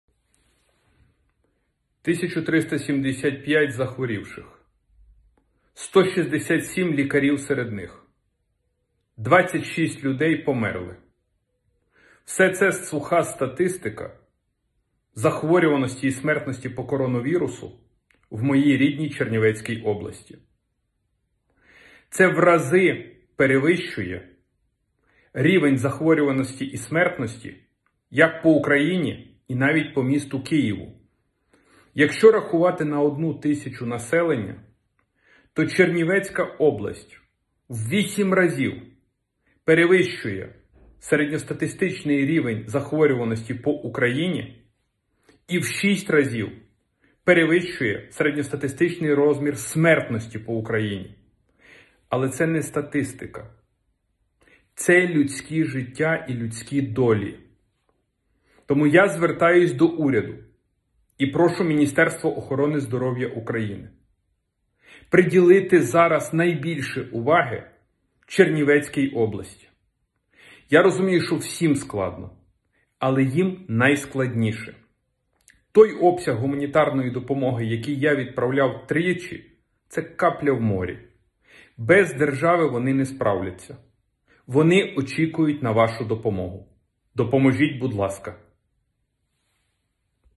Звернувся до уряду і МОЗ приділити увагу Чернівецькій області через ситуацію з коронавірусом
У моїй рідній Чернівецькій області рівень захворюваності на коронавірус на тисячу людей у 8 разів перевищує середній по Україні, а смертність - у 6 разів вища.

Для когось це - суха статистика. Але це людські життя і людські долі.

Звертаюсь до уряду і МОЗ: потрібна державна допомога Чернівецькій області.